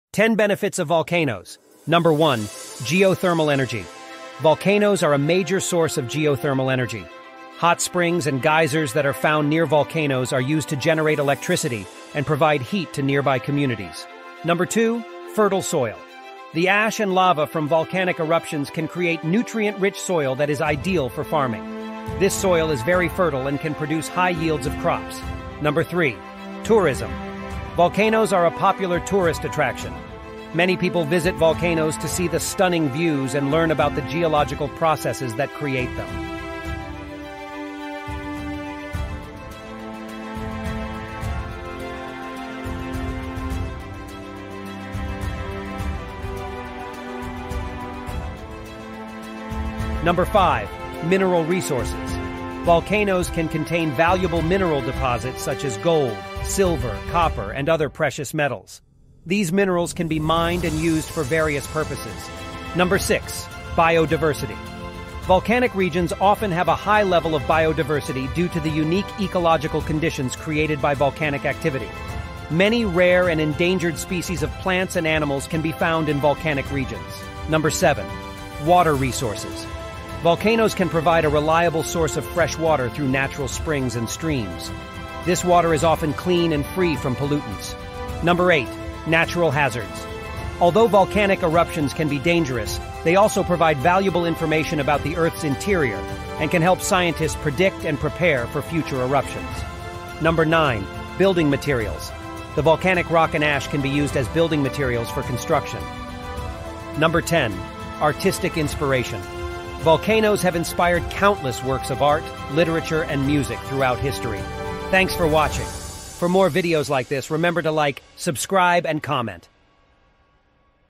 🌋10 Benefits of Volcanoes What's your favorite? Comment 👇
@pinkhatcrypto365 Volcano eruptions are on the rise! When a Volcano erupts it's caused by a build-up of gas pressure in the magma chambers! Boy is it explosive! A volcano bay of lava and ashes fills the atmosphere! Check out the 10 Benefits of volcanoes. What's your favorite benefit? Comment.